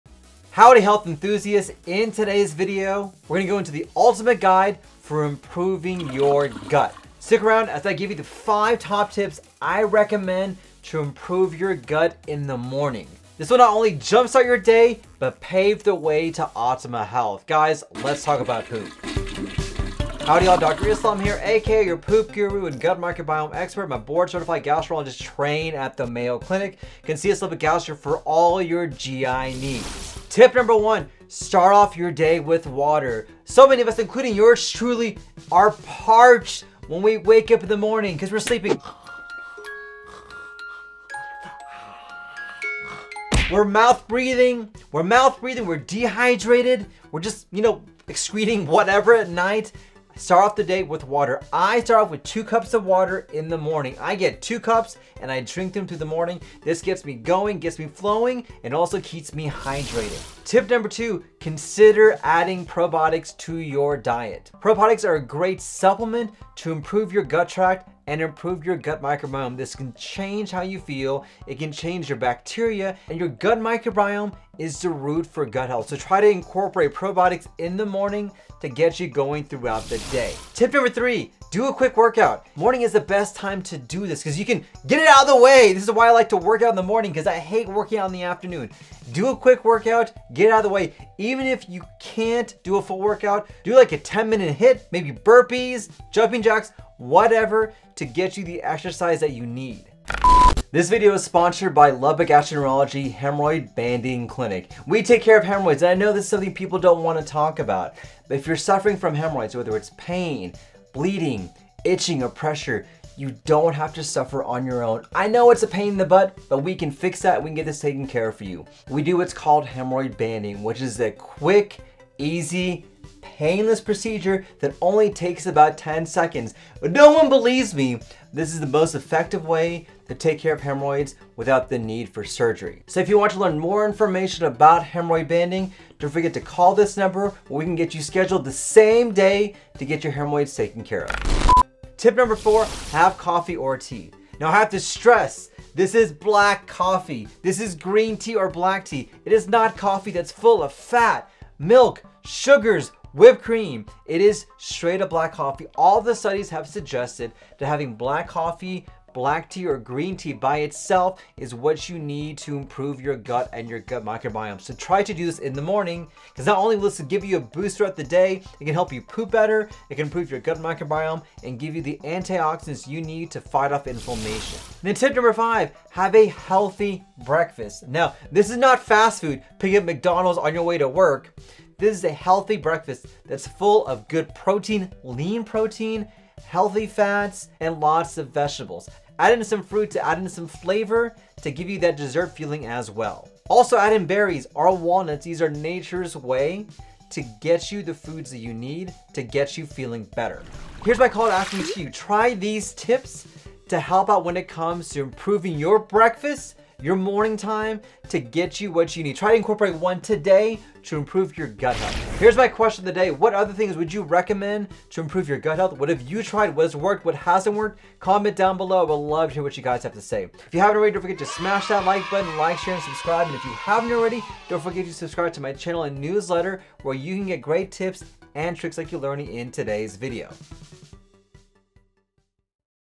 Morning Digestion Tips to Ease bloating and Improve Your Health
Looking to ease your bloating? Here are doctor tips for morning rituals to ease your bloating and improve your health!

Chapters:
0:29 Water
0:59  Probiotics
1:19 Workout
2:21 Coffee/Tea
2:53 Healthy Breakfast
3:19 CTA